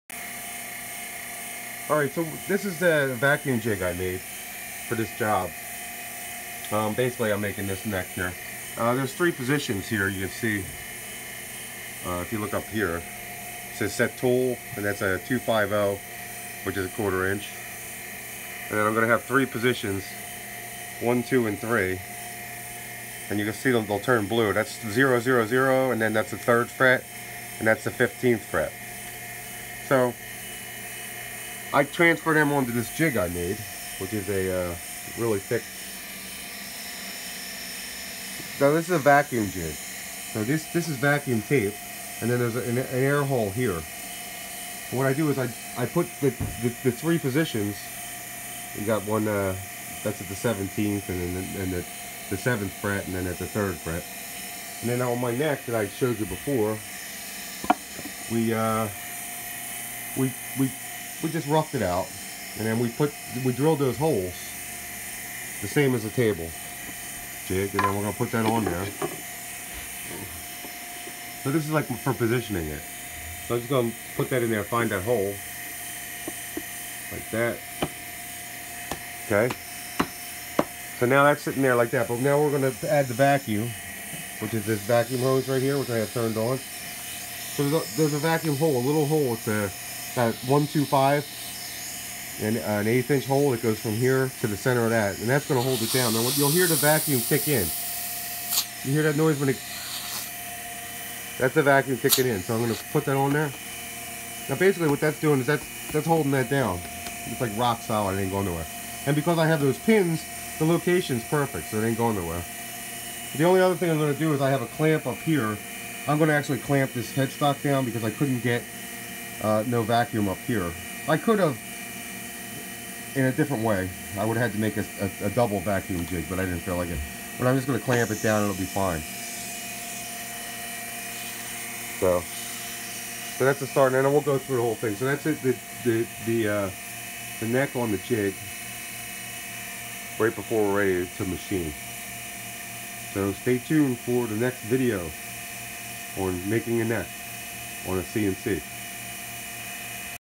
How to make a vacuum jig to hold a guitar neck
This video explains the basics of making a vacuum jig to help a guitar neck on a cnc.